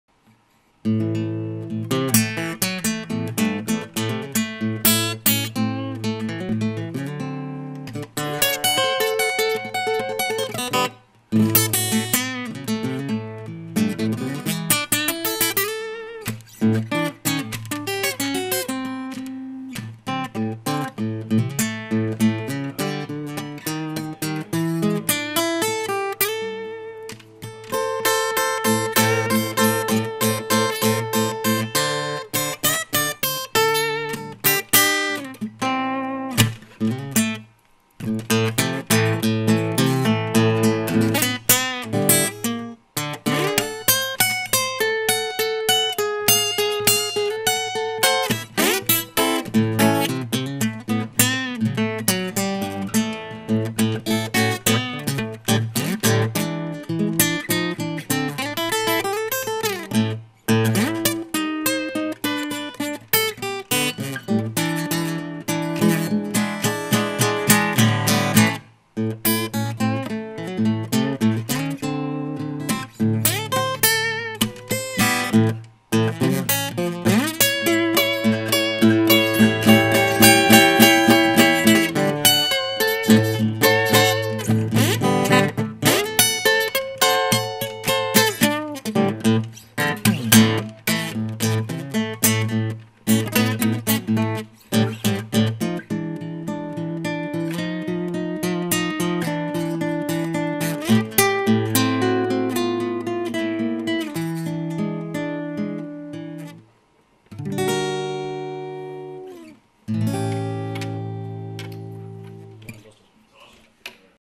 Padraig O´Toole - Acoustic Blues
Irish Singer/Songwriter and Guitarist Padraig O´Toole performing a nice acoustic blues